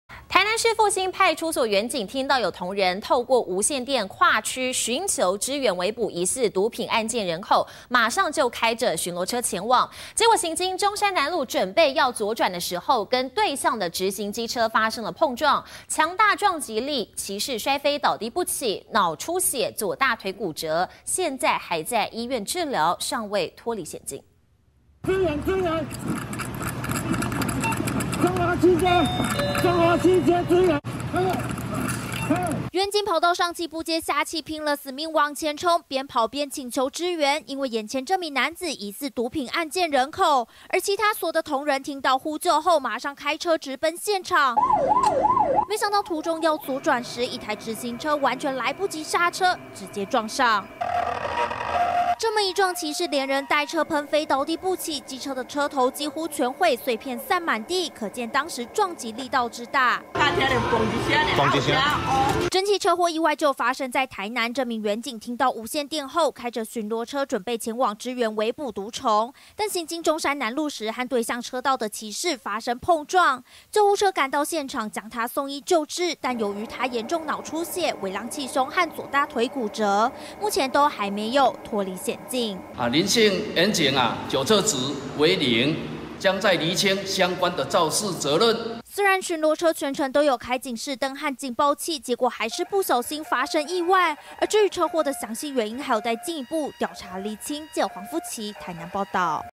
巡邏車支援圍捕毒蟲 和直行騎士發生碰撞
台南市復興派出所員警，聽到有同仁，透過無線電跨區尋求支援圍捕疑似毒品案件人口，馬上開巡邏車前往，行經中山南路準備左轉時，跟對向直行機車發生碰撞，強大撞擊力，騎士摔飛倒地不起，腦出血，左大腿骨折，目前還在醫院治療，尚未脫離險境。